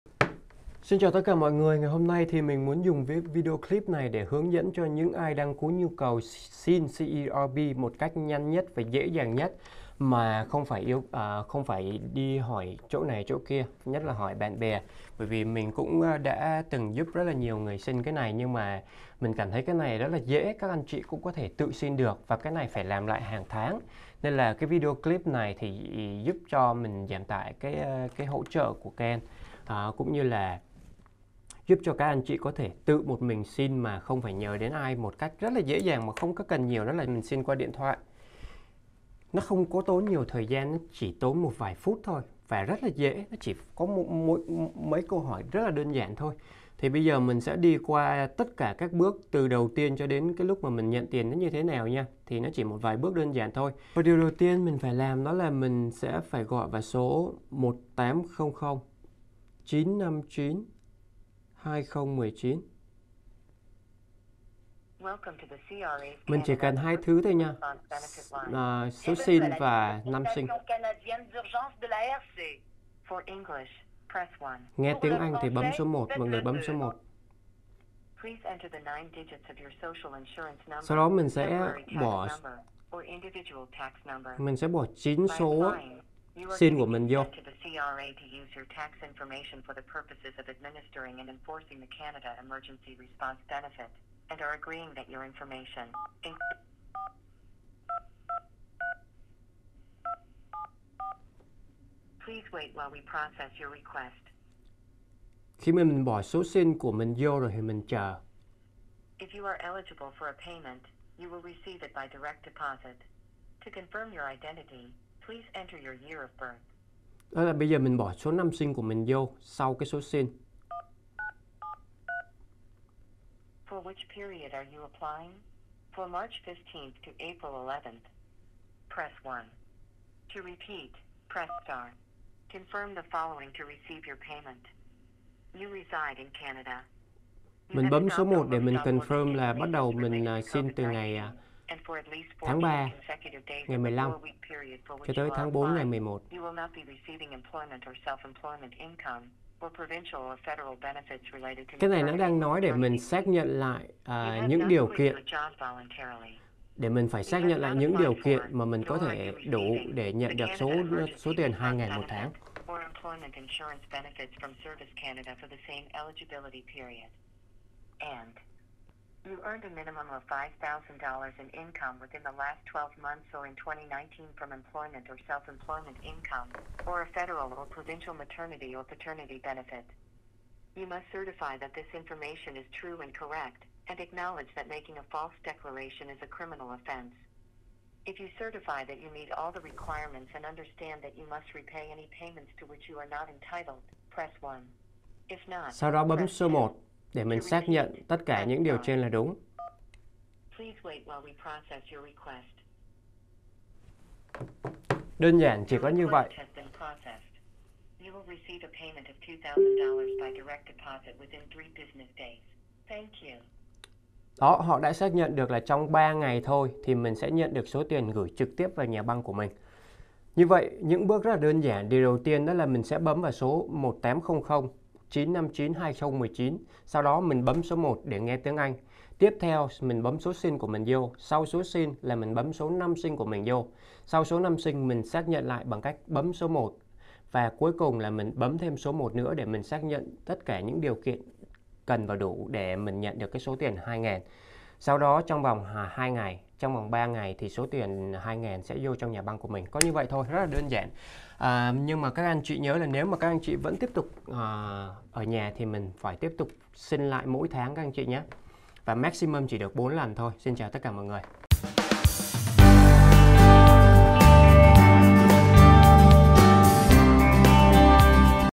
Xin CERB (Canada Emergency Response Benefit) qua Điện Thoại 1 cách Đơn Giản Nhất
Cho những ai:
* Đủ Điều Kiện
* Muốn xin quan Điện Thoại
* - Sinh tháng 1,2,3 sẽ apply vào các ngày thứ Hai
- Sinh tháng 4,5,6 thì apply vào các ngày thứ Ba.
- Sinh tháng 7, 8, 9 thì apply vào các ngày thứ Tư.
- Sinh tháng 10, 11, 12 thì apply các ngày thứ Năm.